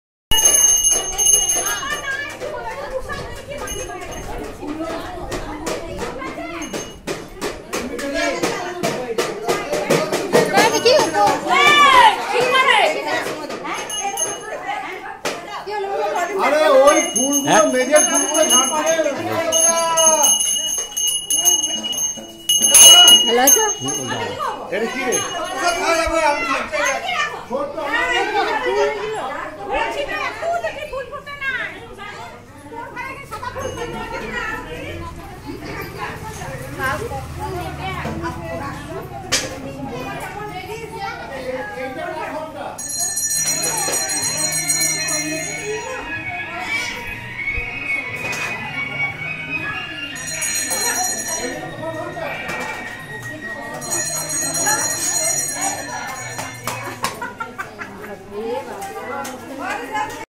Durga Puja # @Rural Bengal# Coursty # Sanjeev Mukherjee🪄 #🎥🎀 Editing* Khasish Barua.
Durga puja#@rural Bengal# 😊🛕🛖🛖Village..Dwariapur, 3km from Guskara municipality toward west.Guskara is a town and a municipality under the Ausgram police station in Bardhaman Sadar North subdivision of Purba Bardhaman district in the Indian state of West Bengal.🚦⛱️🚦🚧😊🙋‍♂️